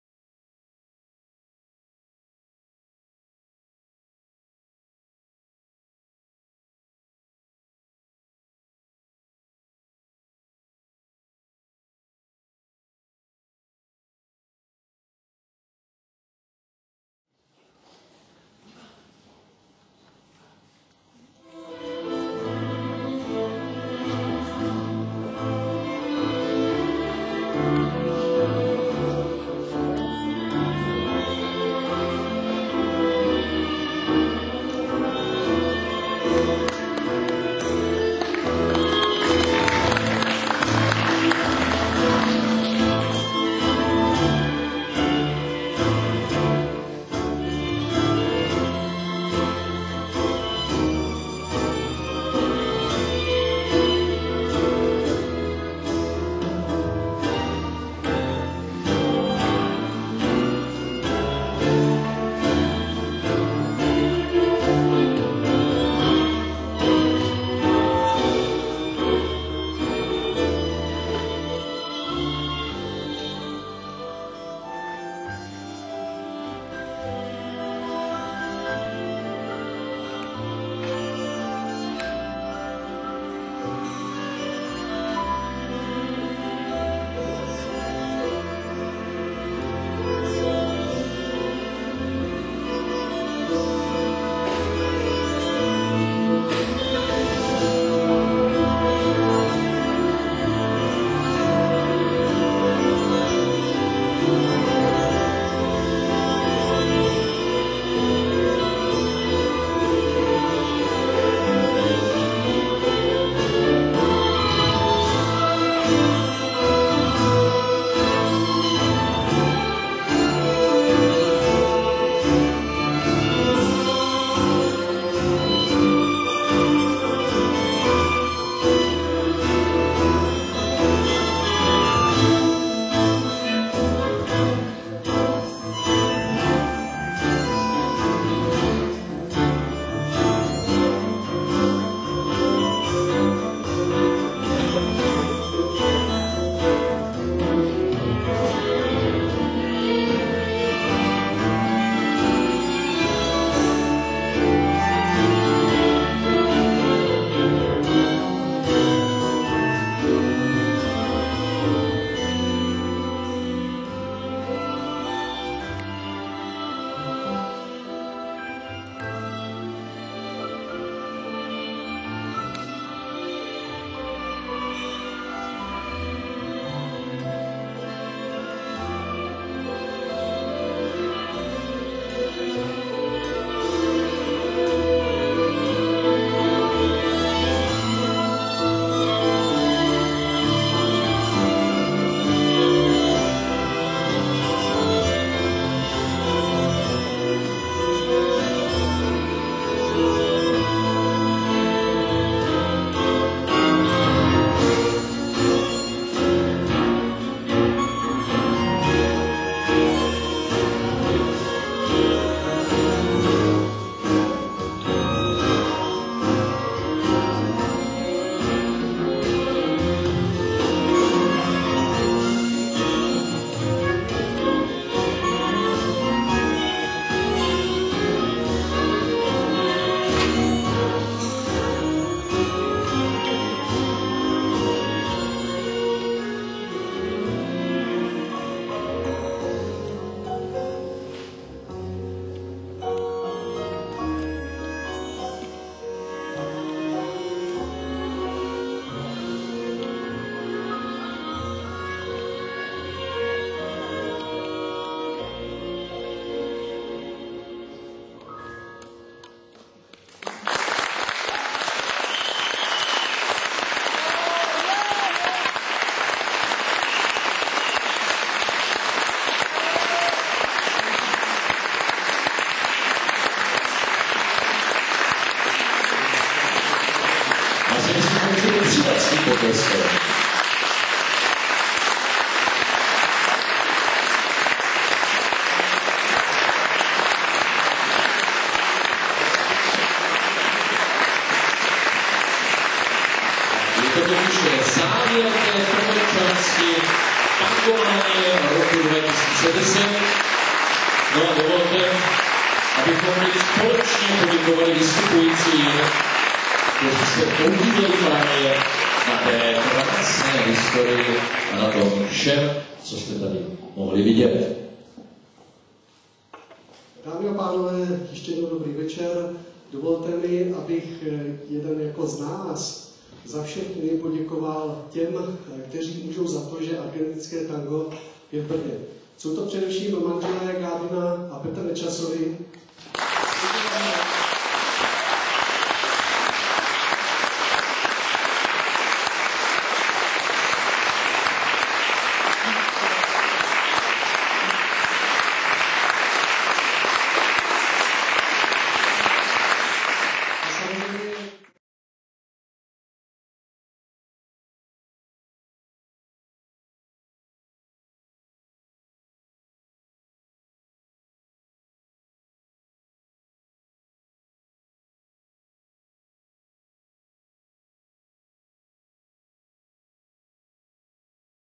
Tangománie II - 9. 10. 2010 - 3. část
večer plný argentinského tanga s hudebním doprovodem Bona Fide Tango Praha, moderoval Vikror Janč, tanec členové sdružení argTANGO.com